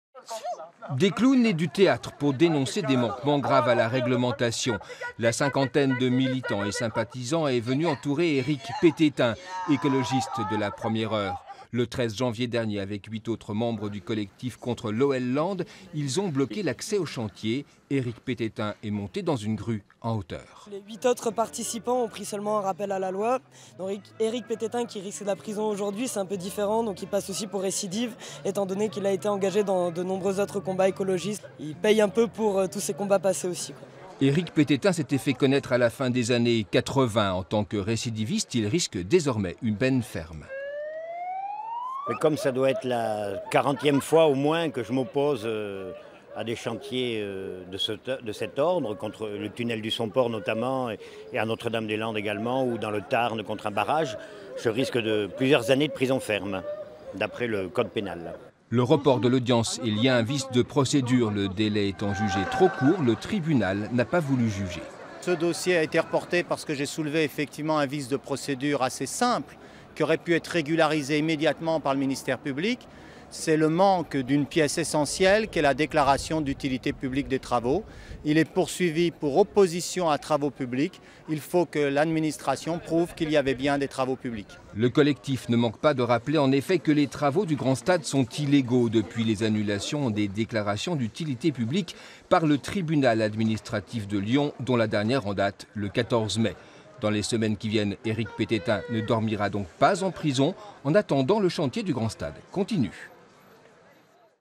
Lyon le procés de Eric Petetin reporté (France 3 région)
Le reportage de D.Pajonk et JE.Gay (montage C.Martin) - 19/05/14 - intervenants : Jenny (Collectif Rhône Notre-Dame-des-Landes) - Eric Pétetin - Etienne Tête (avocat d'Eric Pététin)